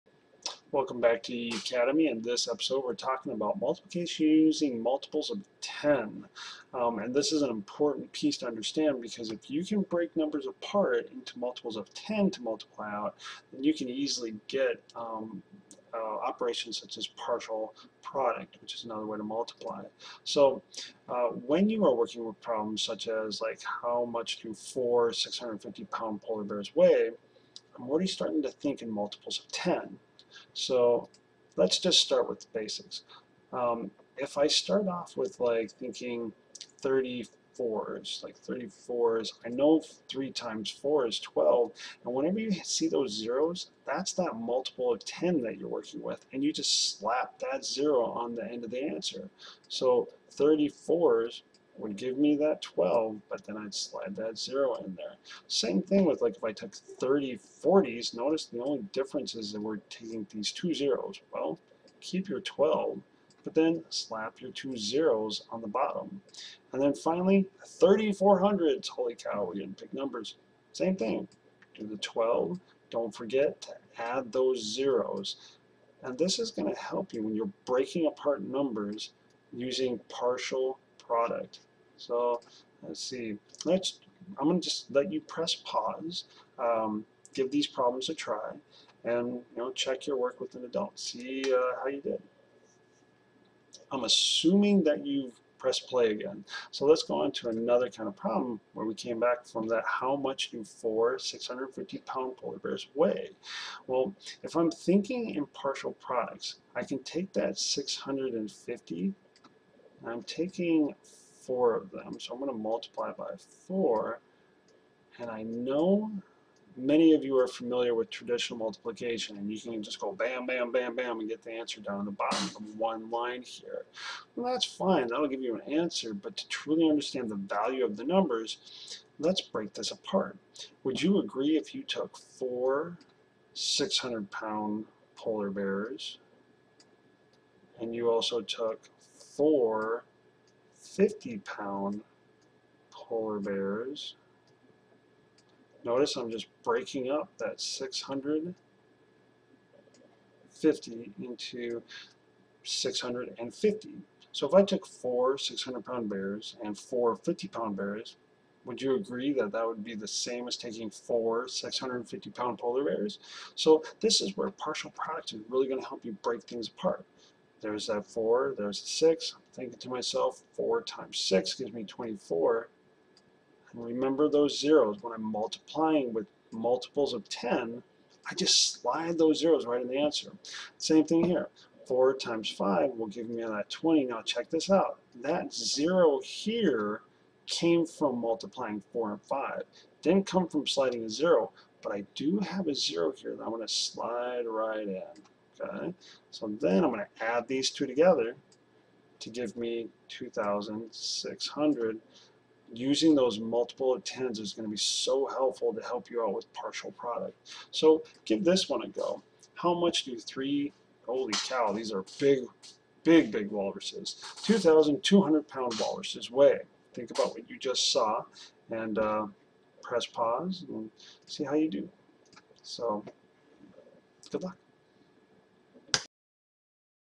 Multiplying Multiples of 10
Multiplying Multiples of 10. Found in Grade 3 Everyday Math Unit 9